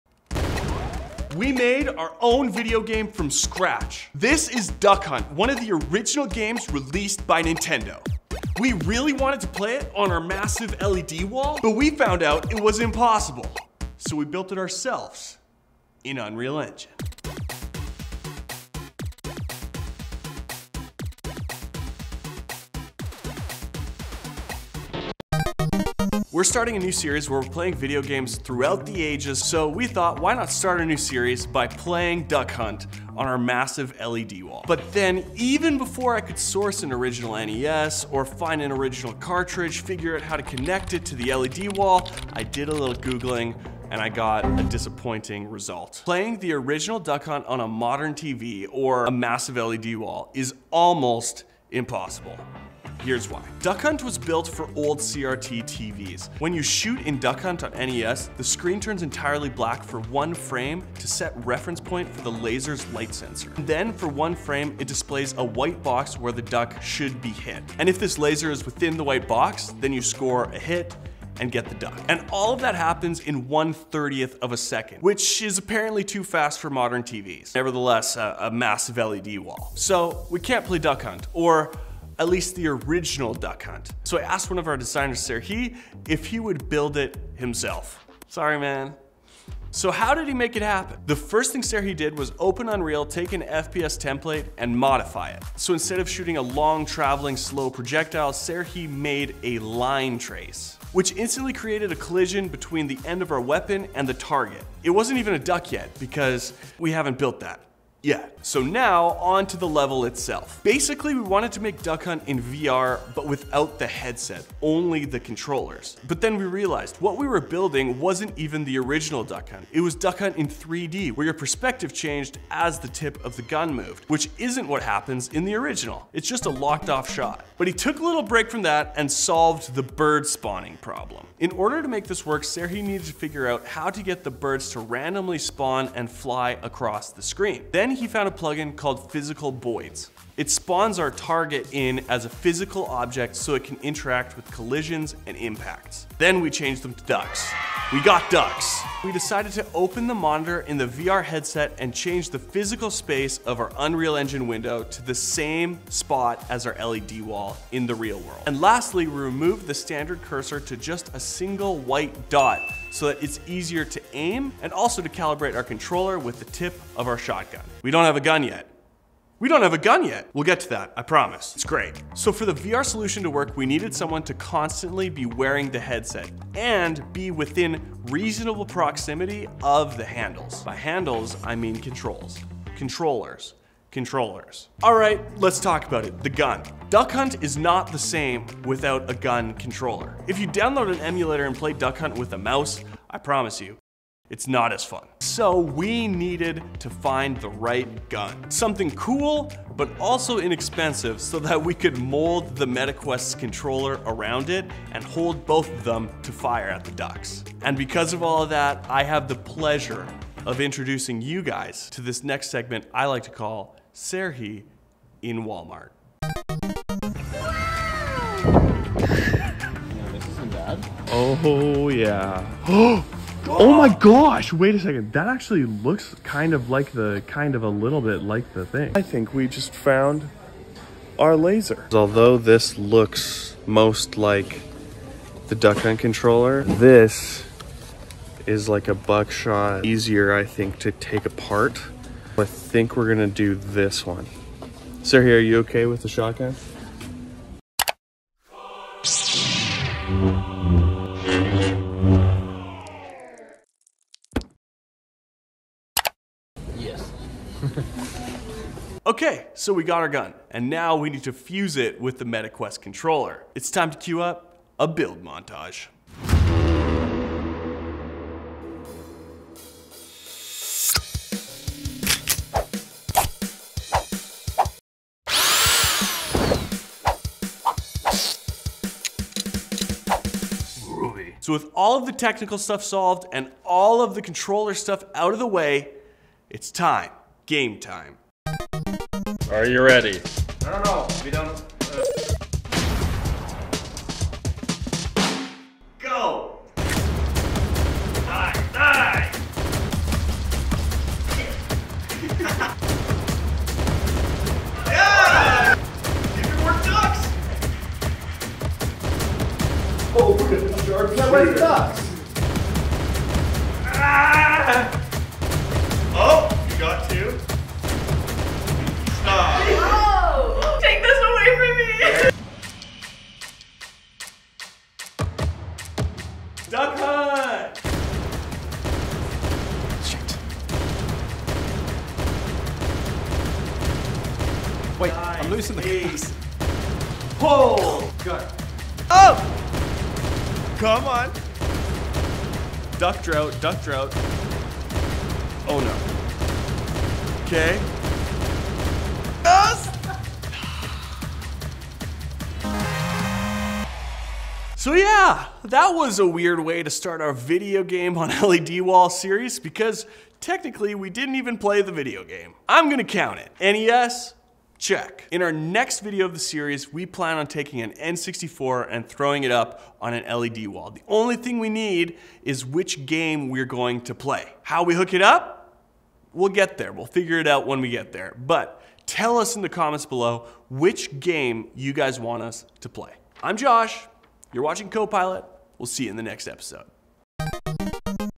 Remaking DUCK HUNT in Unreal Engine 5
We built Duck Hunt for NES from scratch in 3D by using Unreal Engine 5! Our studio has a large LED wall that we want to play games on, and what better way to start this series than build our own game from scratch?!